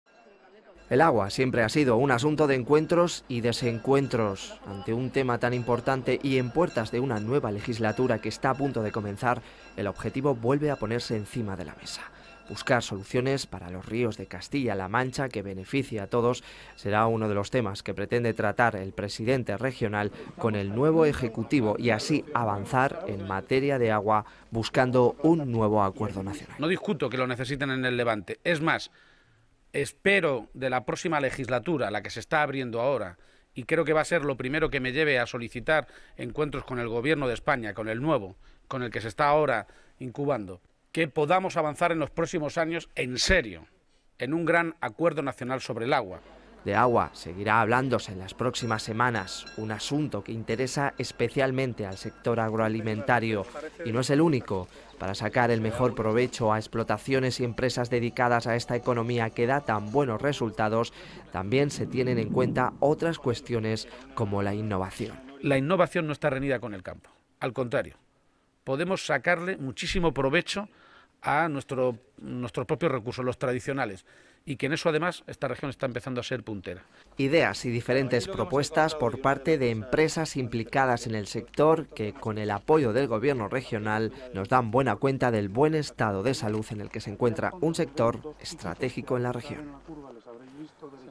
PAGE PLANEA UN ENCUENTRO CON EL GOBIERNO CENTRAL PARA TRATAR EL TEMA DEL AGUA
De agua volvemos a hablar, y de su distribución. El presidente de Castilla-La Mancha, Emiliano García-Page, solicitará un encuentro con el ejecutivo central para encontrar solución a un tema que preocupa, especialmente al sector agroalimentario.